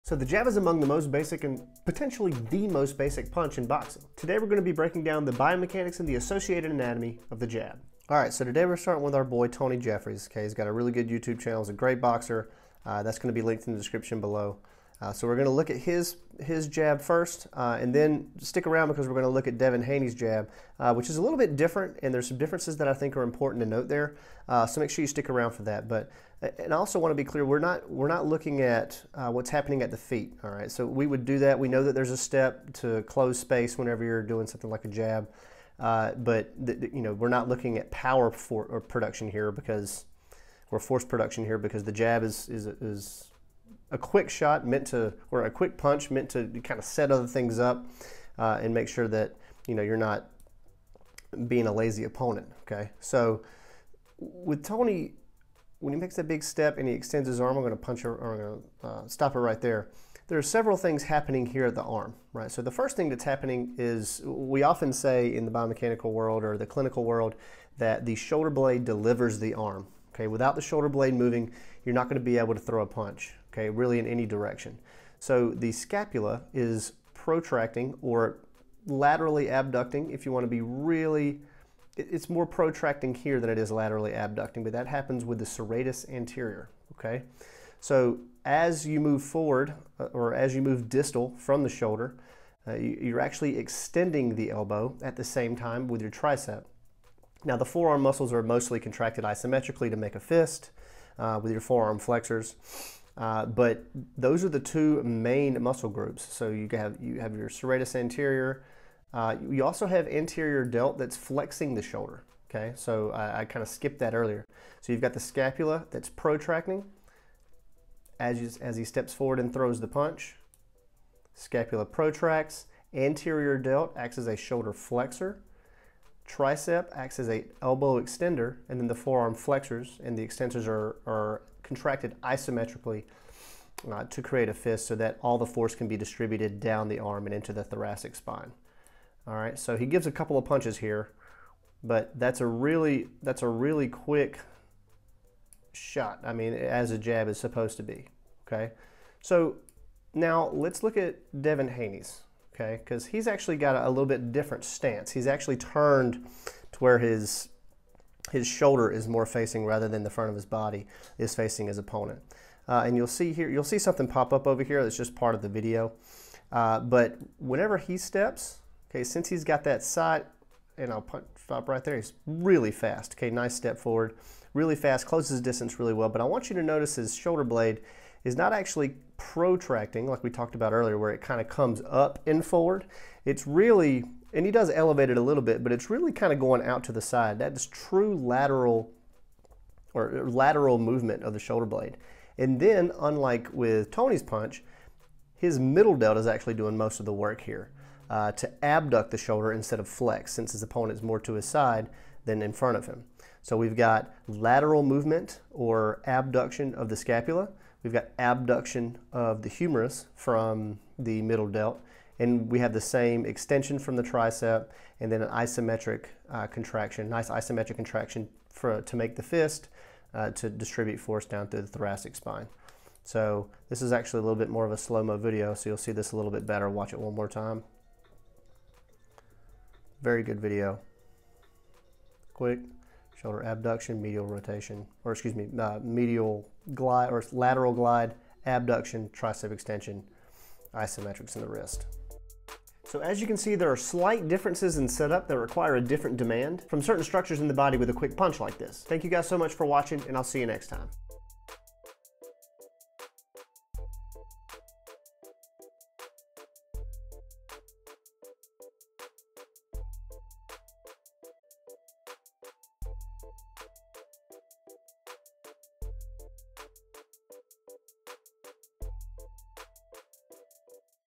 Biomechanical Analysis of Devin Haney’s and Tony Jeffries’s Jab
This video breaks down the biomechanics of the jab using demonstrations from Tony Jeffries and Devin Haney.

HUGE THANKS to the Human Anatomy Atlas App by Visible Body for the AMAZING quality!